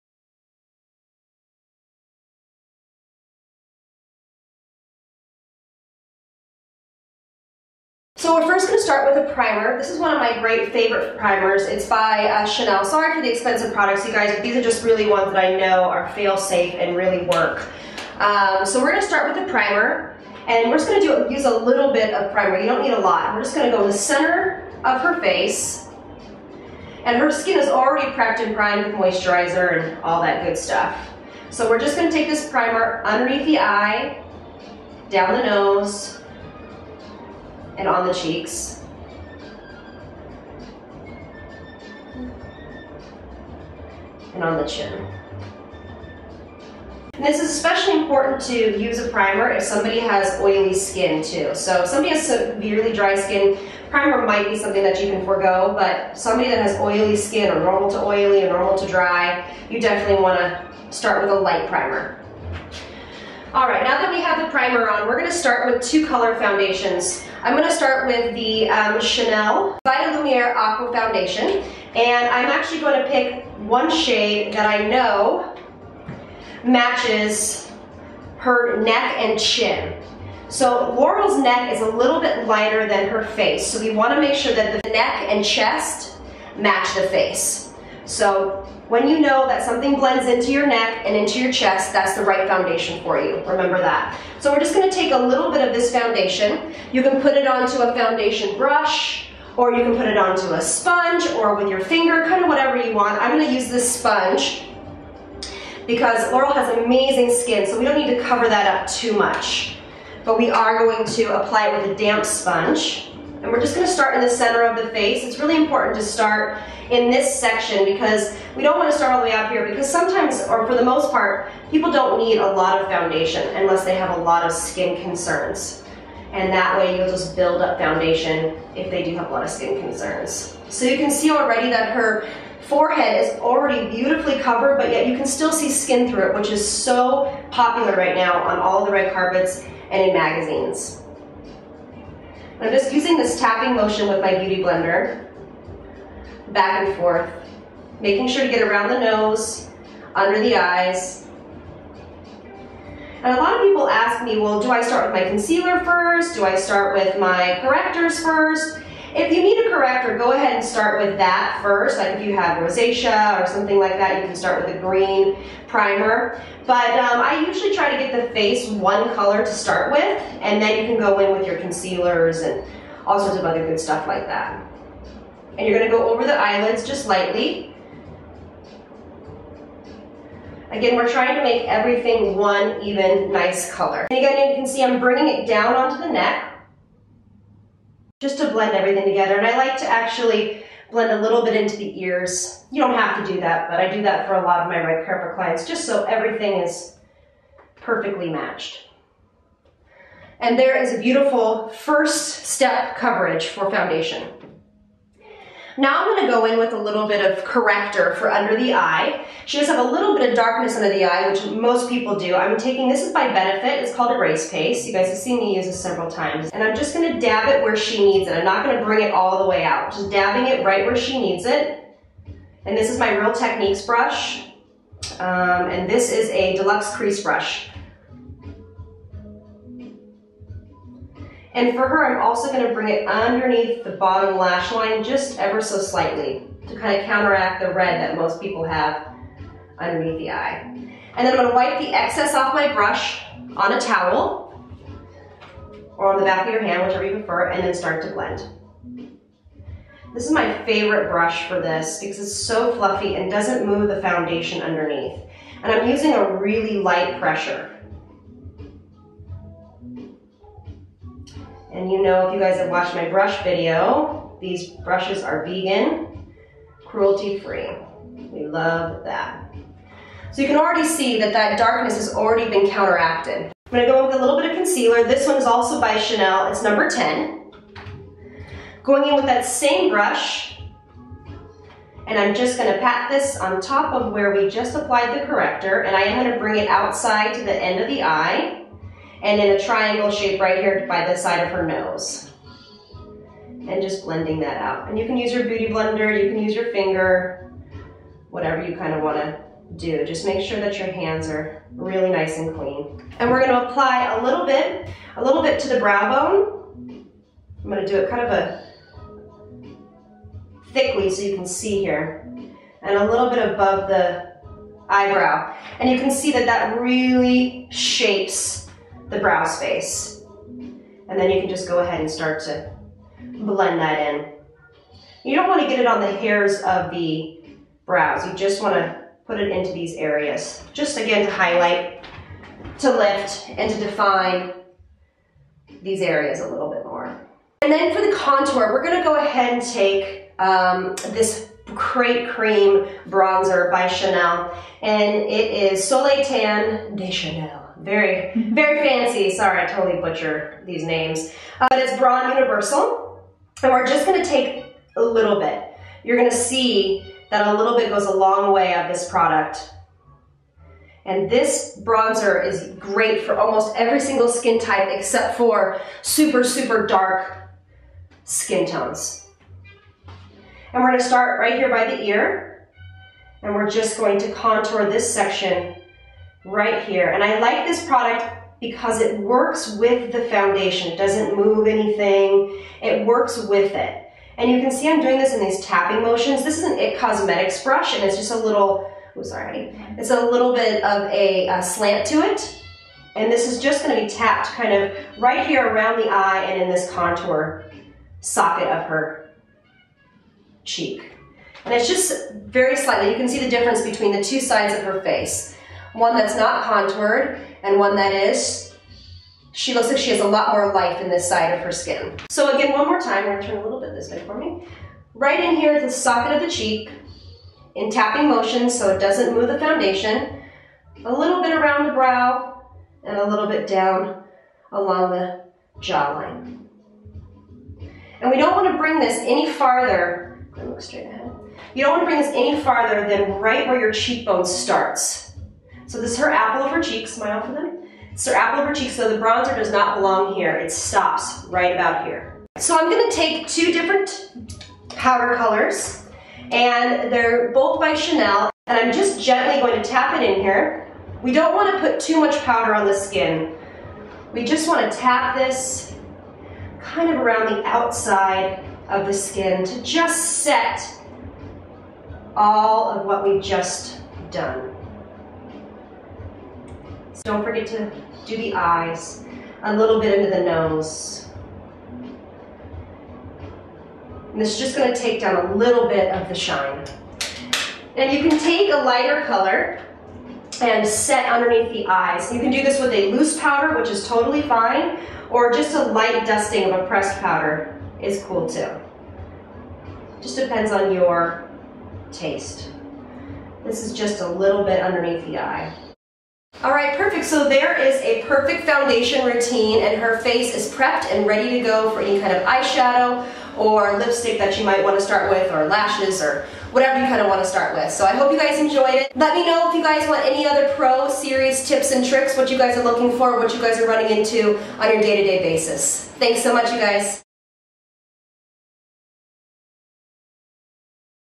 Natural Foundation Routine- Pro Series
Hey guys!!
So here is my next video in my pro series. This video is for a natural foundation routine for clients that don't need too much skin correction. This look is also perfect for your red carpet clients. Let me know what other video's you want me to do in this series!! Remember everyone these are just general rules and might not work for every client. So play around with what works for you.